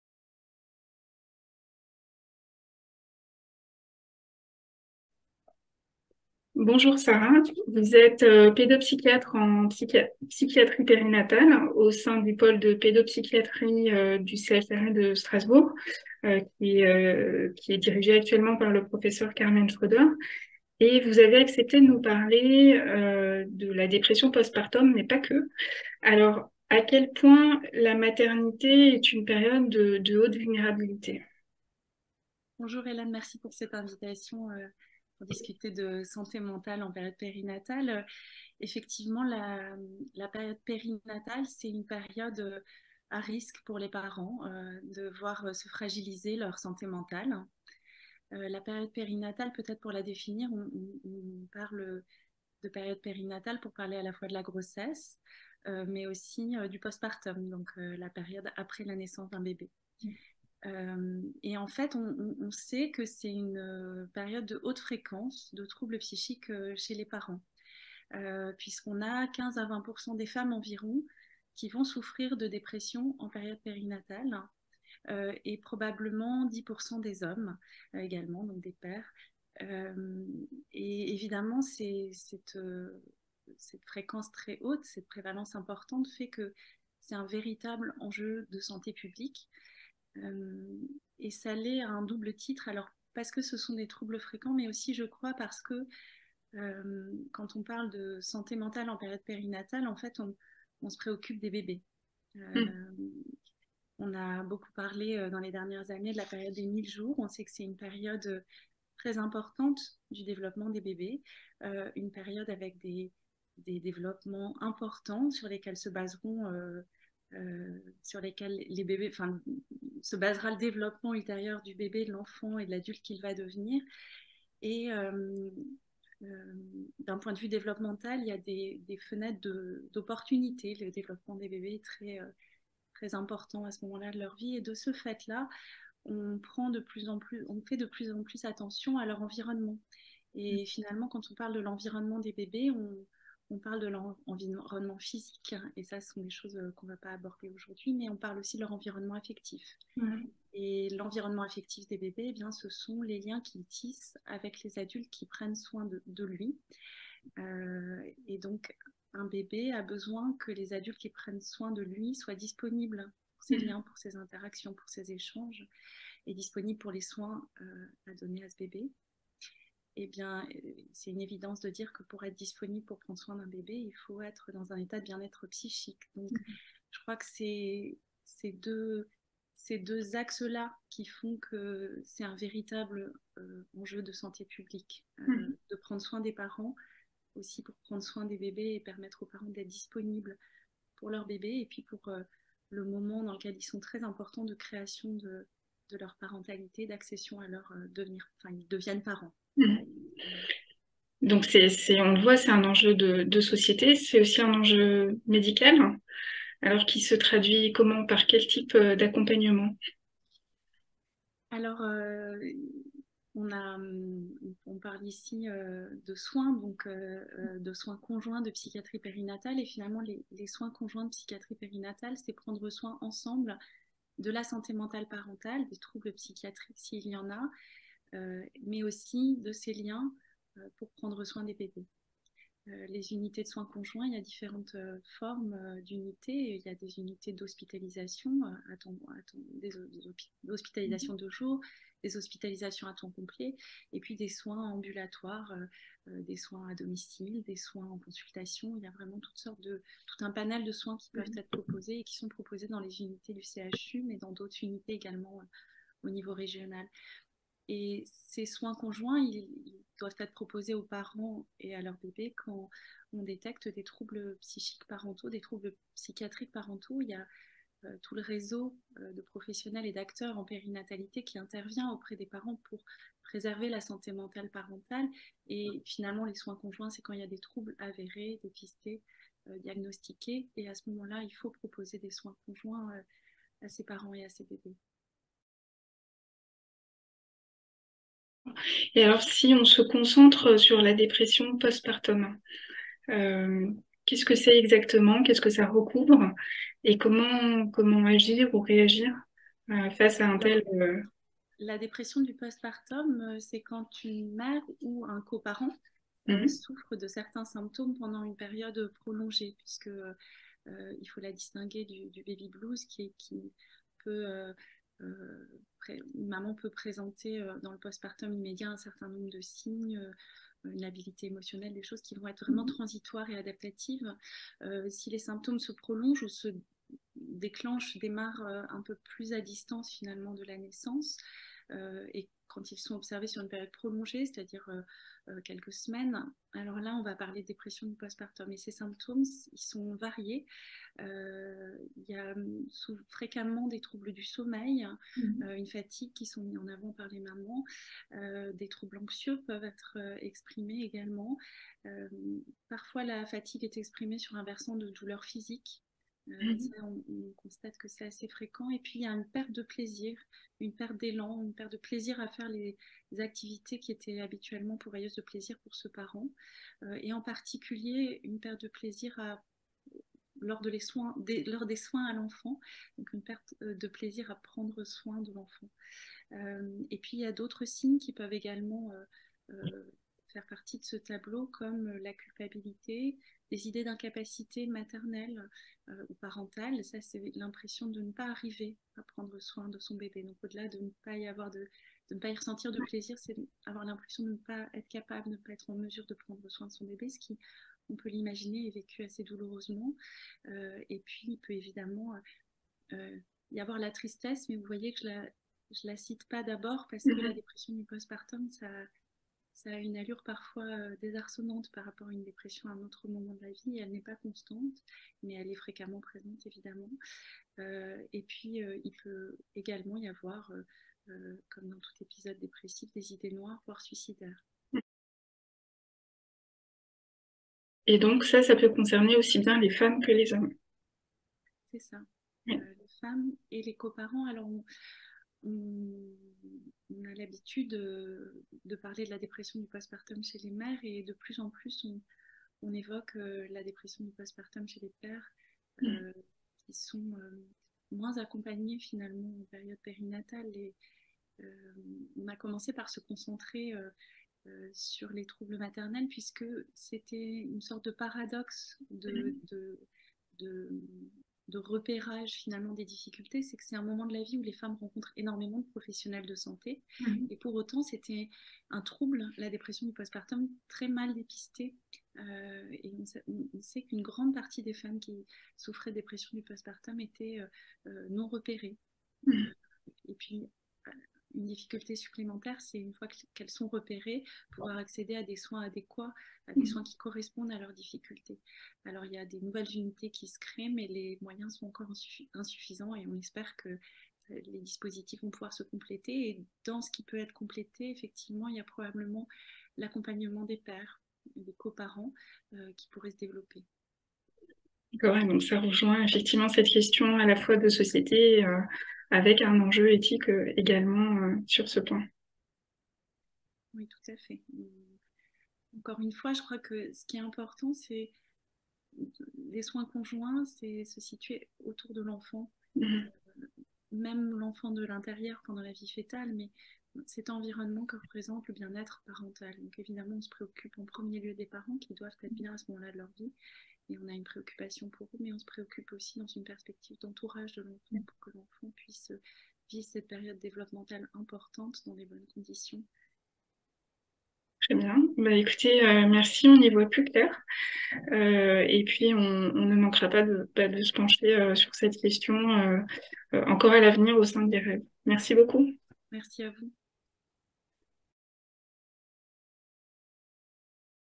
Qu'est-ce que la périnatalité ? Par Dr Sarah Sananes
Le Dr Sarah Sananès, psychiatre en psychiatrie périnatale du CHRU de Strasbourg, nous éclaire sur le sujet de la périnatalité, de la dépression post-partum et des enjeux de soin sur cette période de grande vulnérabilité.
Réalisé par Hélène Gebel, Coordinatrice régionale de l'EREGE